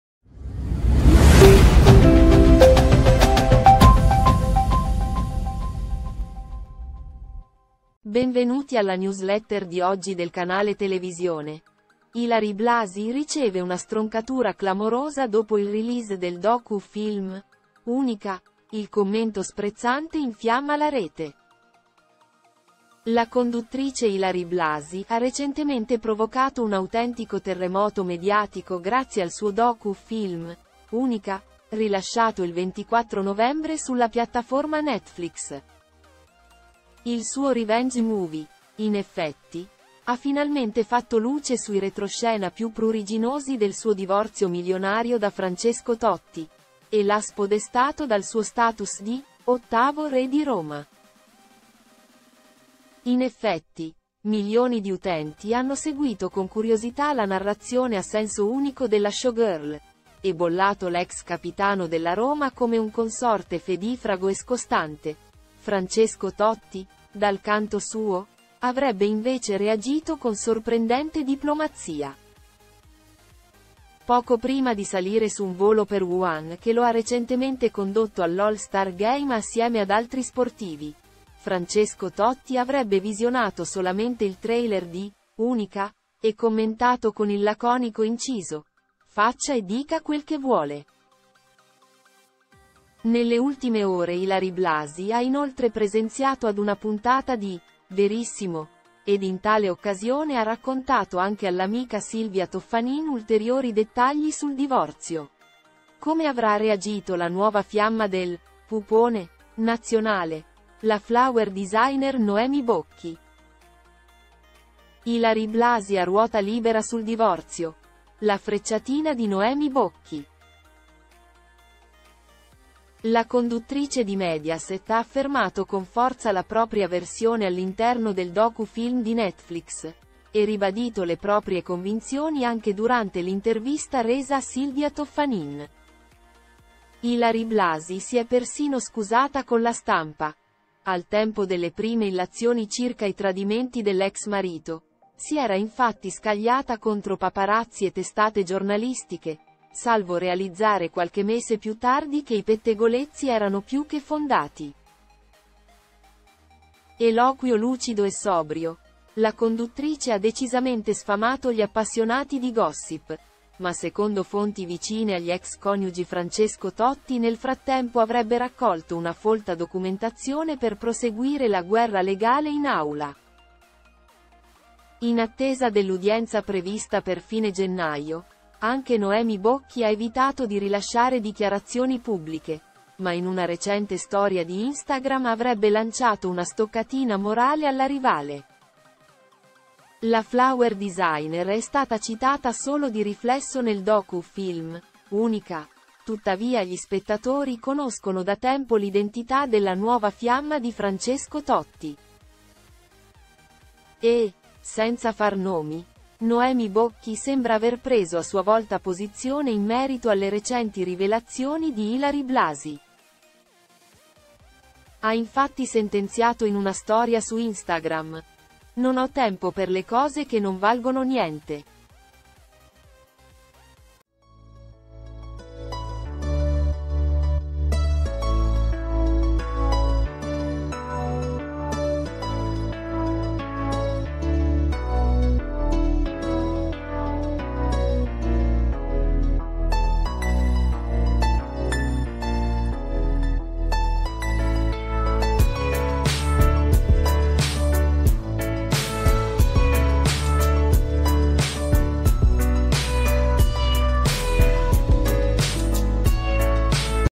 “Non vali niente” Ilary Blasi minacciata dalla sua più acerrima nemica
La conduttrice Ilary Blasi ha recentemente provocato un autentico terremoto mediatico grazie al suo docu-film “Unica”, rilasciato il 24 novembre sulla piattaforma Netflix.

Il suo revenge-movie, in effetti, ha finalmente fatto luce sui retroscena più pruriginosi del suo divorzio milionario da Francesco Totti, e l’ha spodestato dal suo status di “ottavo re di Roma“.

In effetti, milioni di utenti hanno seguito con curiosità la narrazione a senso unico della showgirl, e bollato l’ex capitano della Roma come un consorte fedifrago e scostante; Francesco Totti, dal canto suo, avrebbe invece reagito con sorprendente diplomazia.

Poco prima di salire su un volo per Wuhan che lo ha recentemente condotto all’All Star Game assieme ad altri sportivi, Francesco Totti avrebbe visionato solamente il trailer di “Unica”, e commentato con il laconico inciso: “Faccia e dica quel che vuole“. Nelle ultime ore Ilary Blasi ha inoltre presenziato ad una puntata di “Verissimo”, ed in tale occasione ha raccontato anche all’amica Silvia Toffanin ulteriori dettagli sul divorzio… Come avrà reagito la nuova fiamma del “Pupone” nazionale, la flower designer Noemi Bocchi?